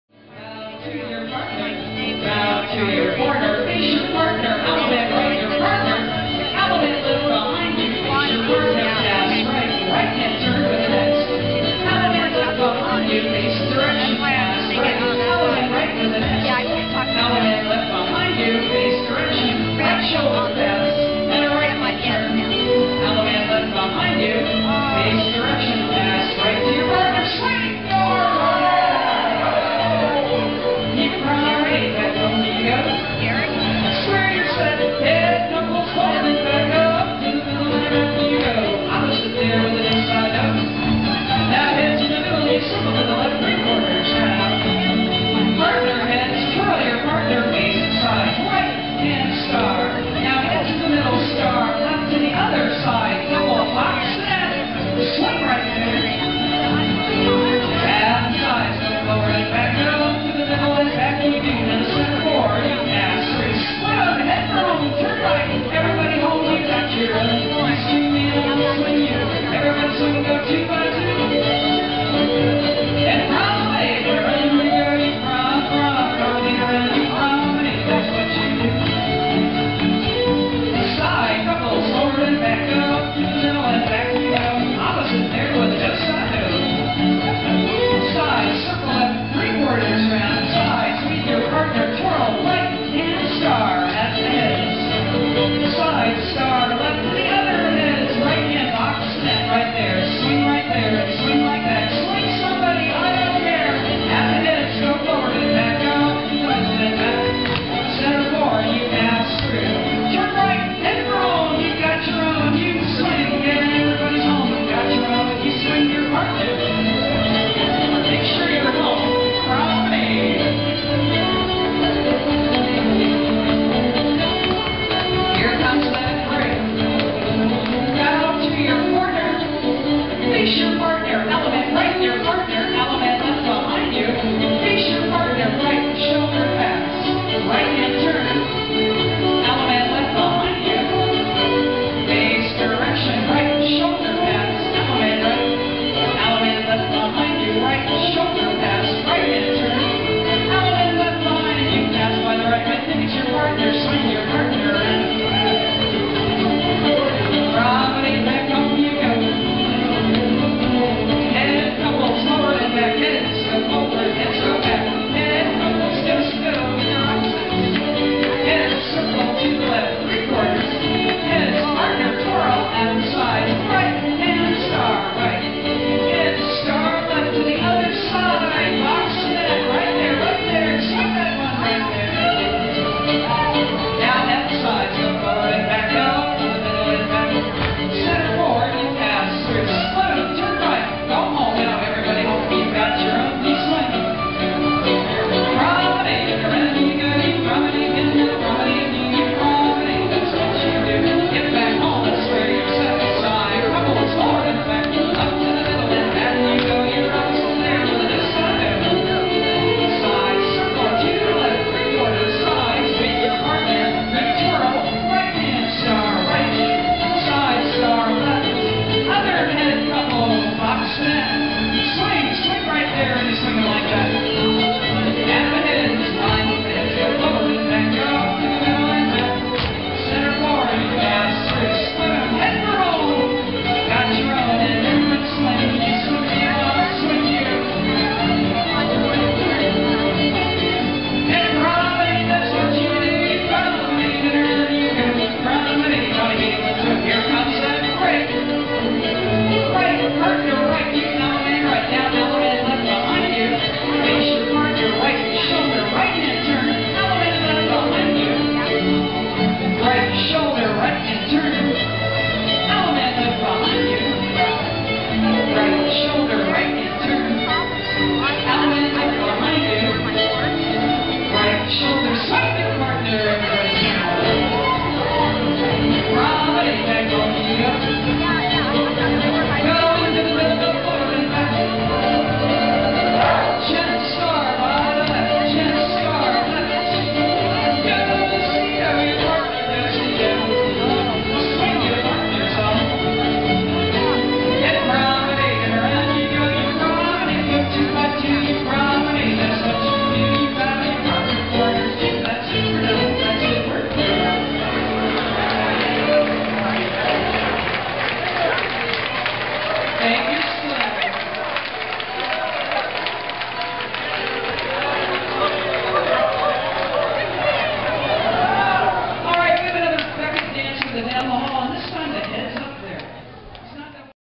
Sat Night Square Fiddling Frog 2010
This is one of the squares that Lisa Greenleaf called. This one has the busy and anticipatory Button Hook Chain, what fun. Jocelyn shot this video from the mezzanine room at the Saturday Night dance of the 2010 Fiddling Frog Contra Dance Weekend in Pasadena California.
Dan Vilter